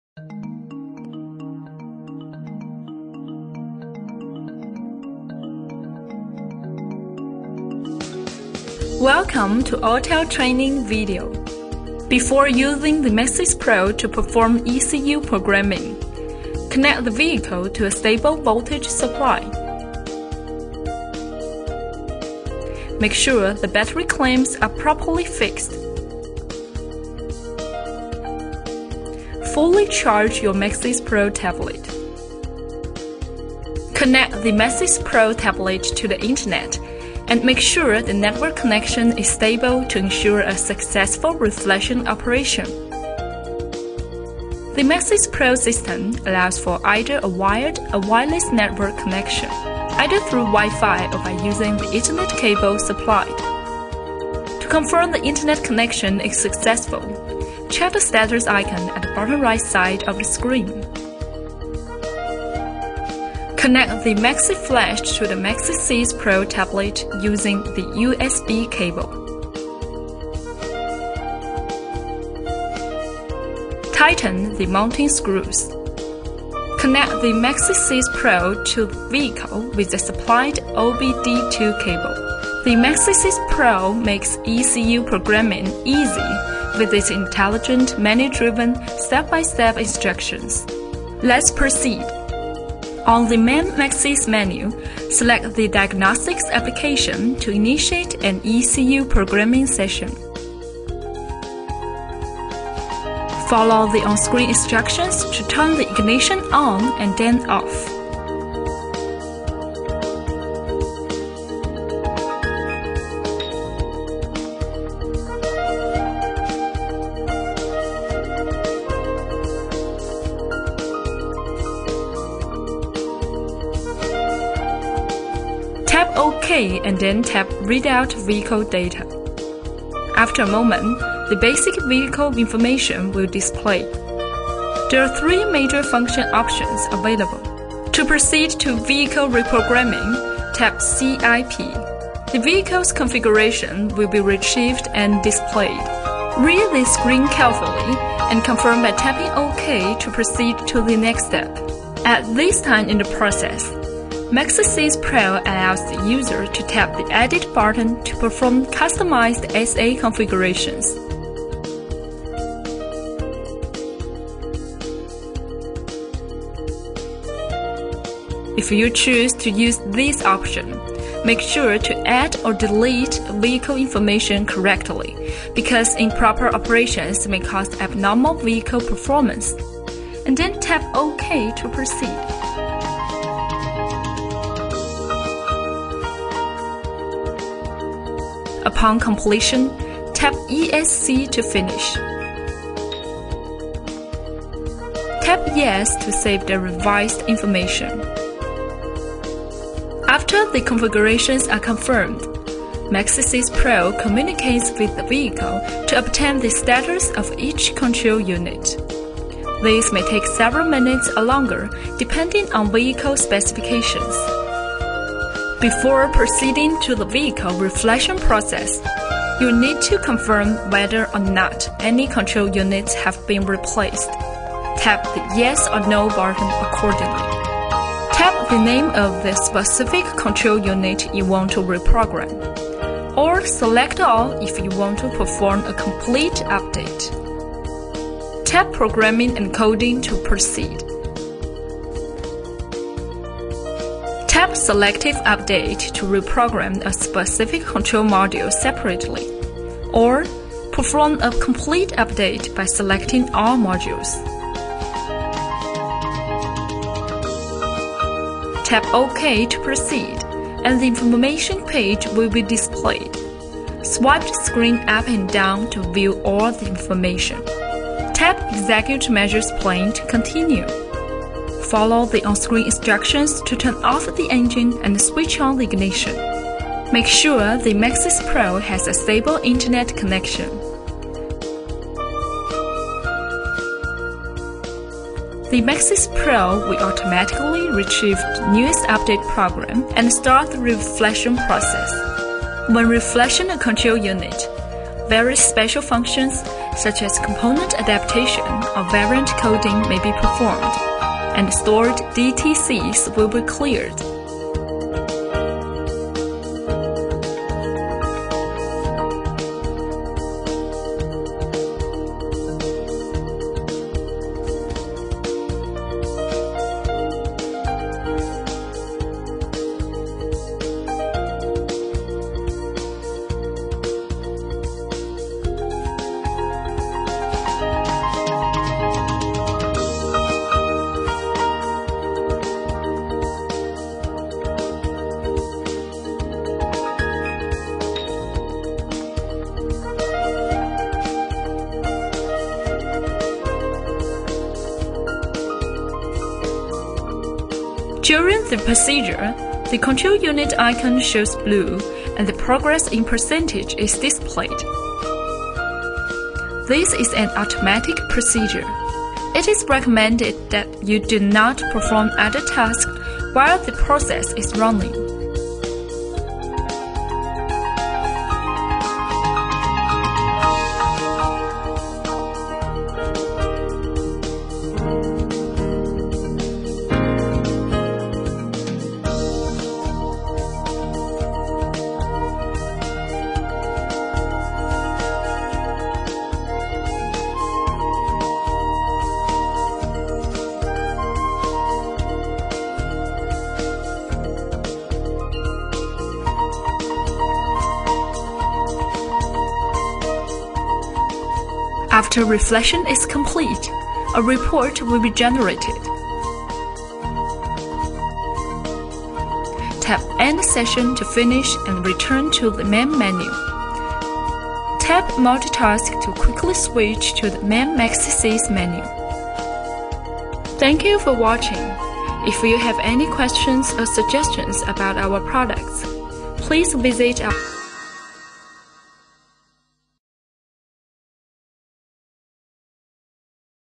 How to use ECU programming on Autel Maxisys Pro
It is an instruction video about ECU programming on Autel Maxisys Pro.